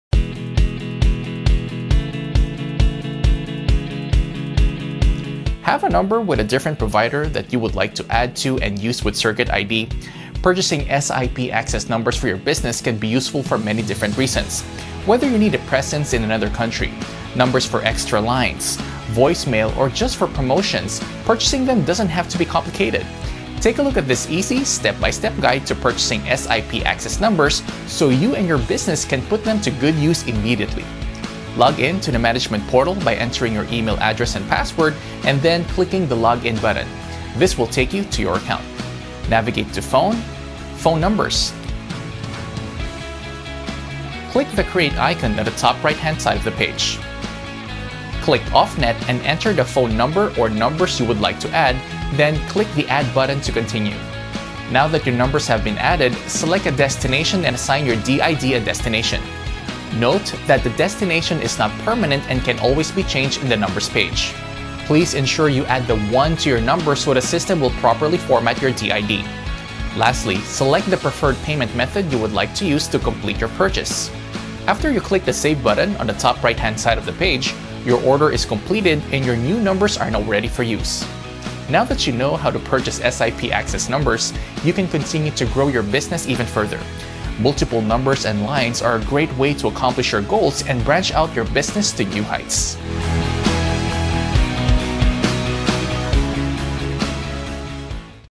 How to Add SIP Access Numbers
This video demonstrates step-by-step instructions for Adding  SIP Access Numbers in the Circuit ID cloud.